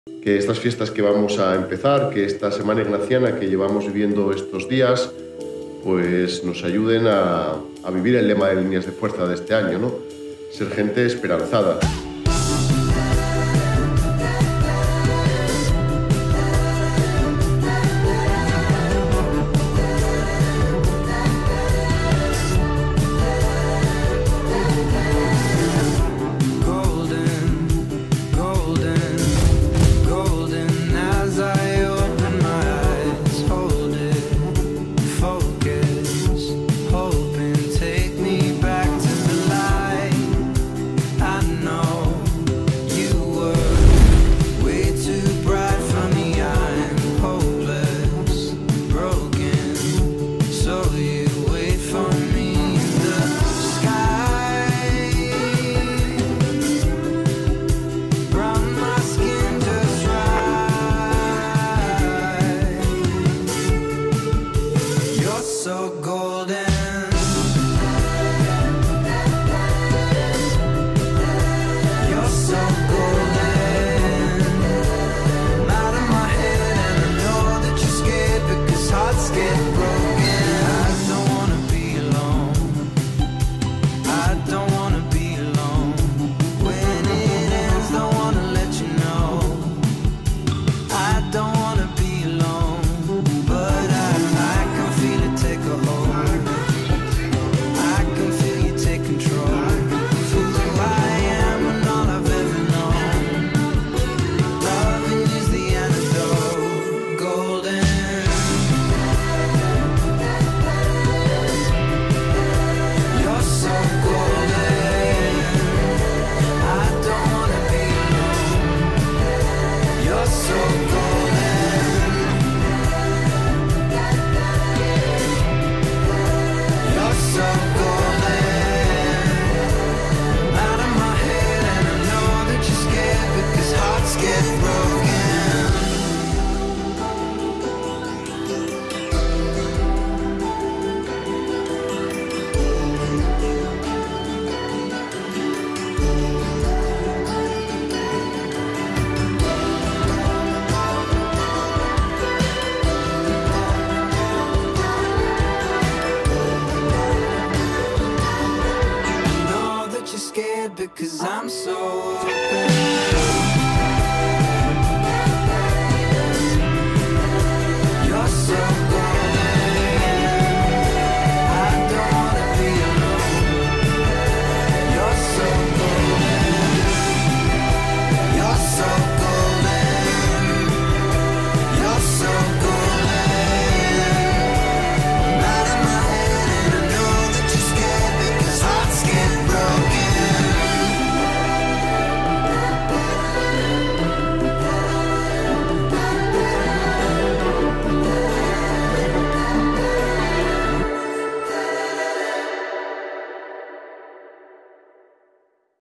Fiestas Sanjo 2023
Esto solo es un pedacito de nuestras queridas fiestas del colegio San José ¡Para entenderlo hay que vivirlo!

Canal oficial del Colegio San José – Jesuitas | Desde 1881 siendo un lugar de acogida, encuentro e innovación en Valladolid.